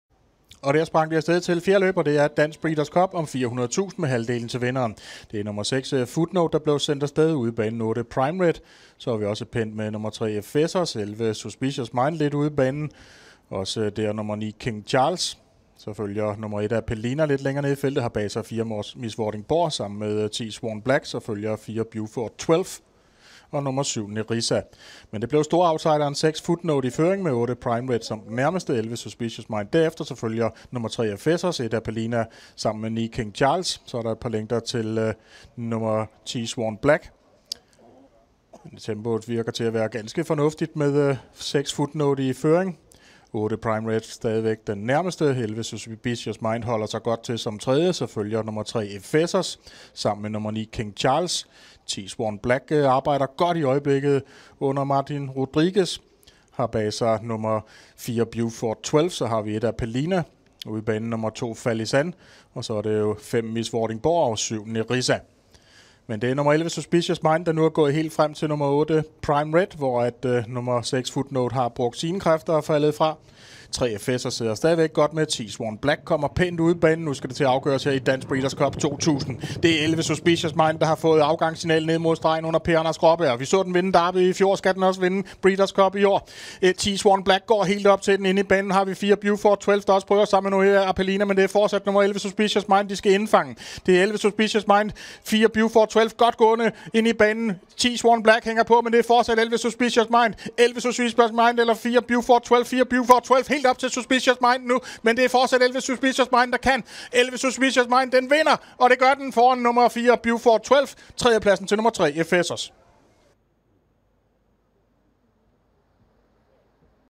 2017.06.25 Dansk Breeders Cup - Suspicious Mind
4yo, Turf 1800 Meters
1.Suspicious Mind (Appel Au Maitre - Fleur En Fleur, by Hernando) Per-anders Gråberg - Niels Petersen 1:50.9
2.Beaufort Twelve
3.Efesos
4.Swan Black
5.Appelina 6.Miss Vordingborg 7.Fali San 8.Prime Red